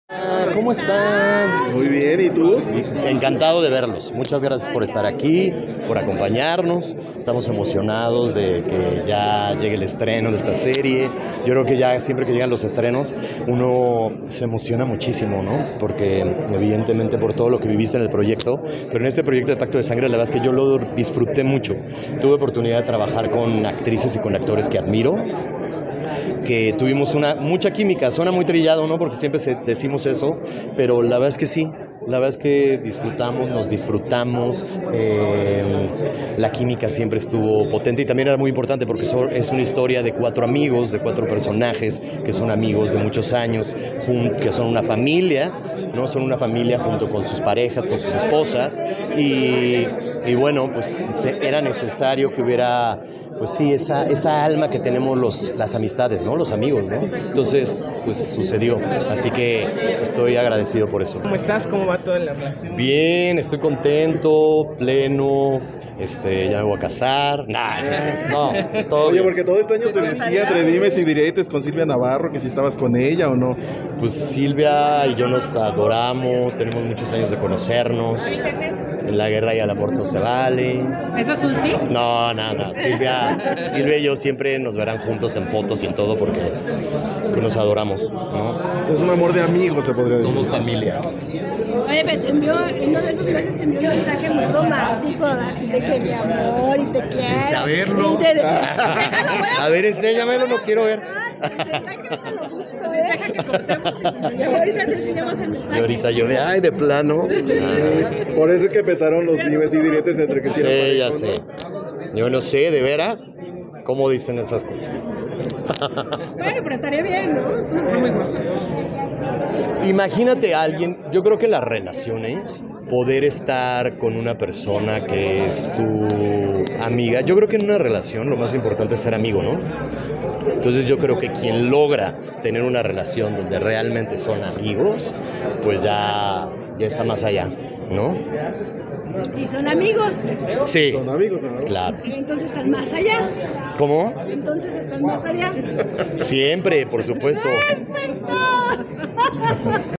Flavio Medina tiene romance con Silvia Navarro?/Estrena Pacto de sangre 🚨💥🔥🚨💥
Flavio Medina.cuwntanainhay eomnce con Silvia Navarro..Checa su respuesta y decide! Estrena Pacto se Sangre.